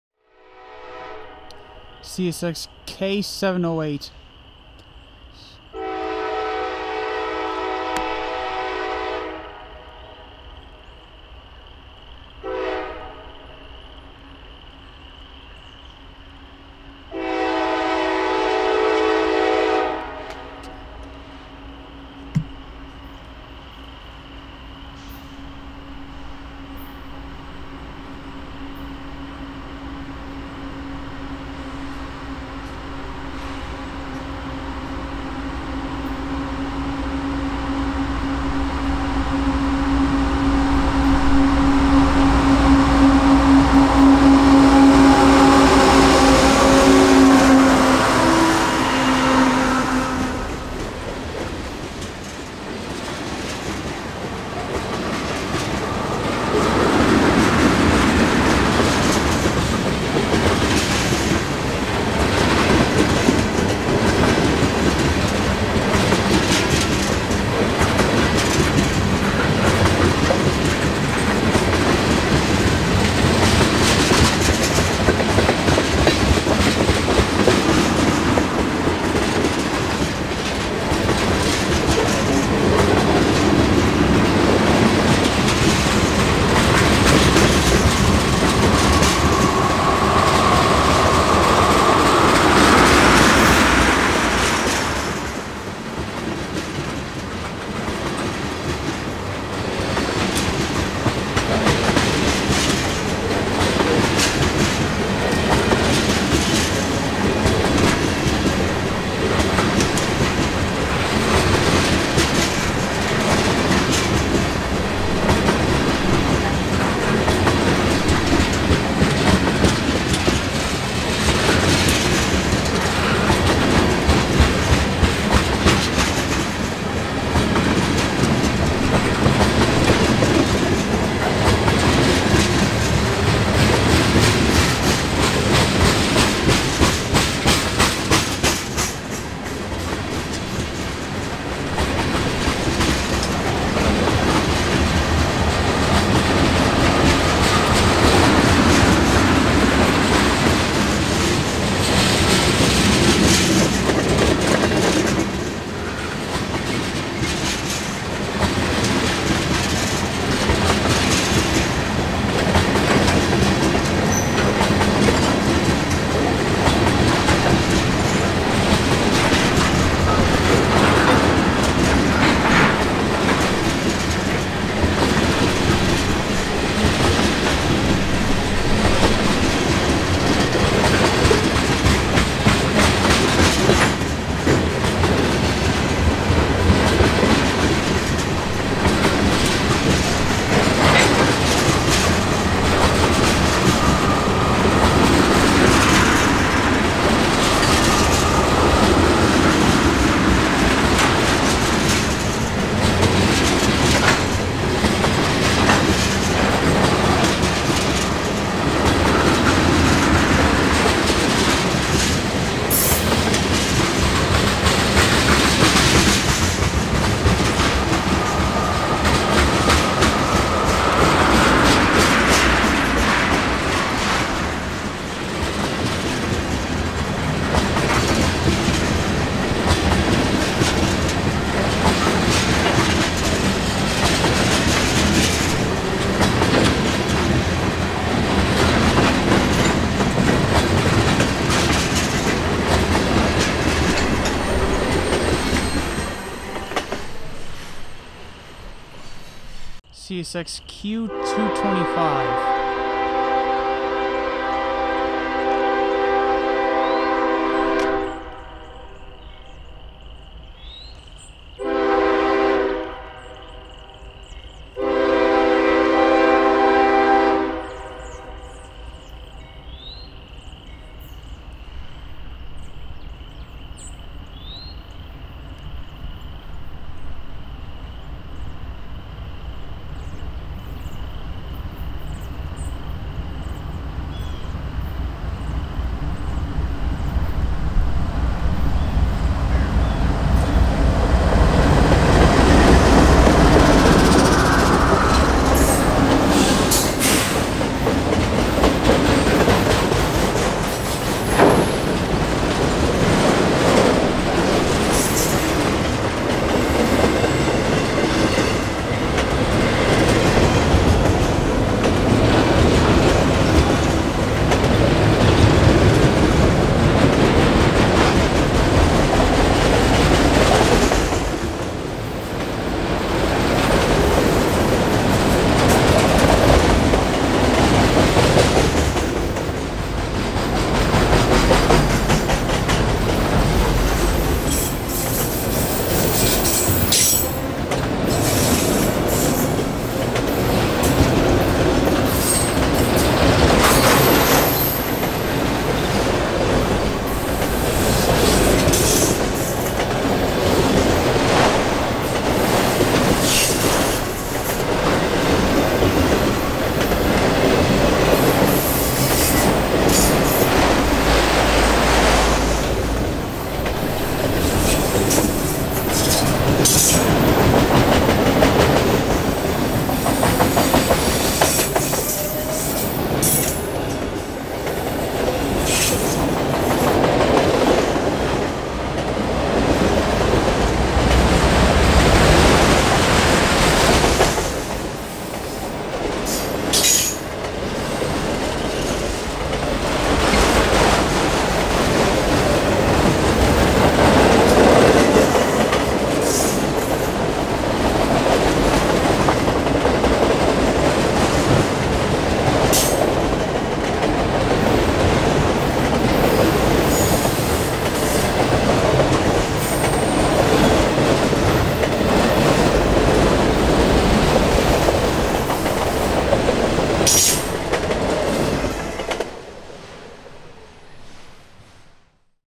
(4K) 2 trains at N E Mcdaniel 2-28-21
2 trains at N.E McDaniel on the CSX S&NA North sub on February 28th 2021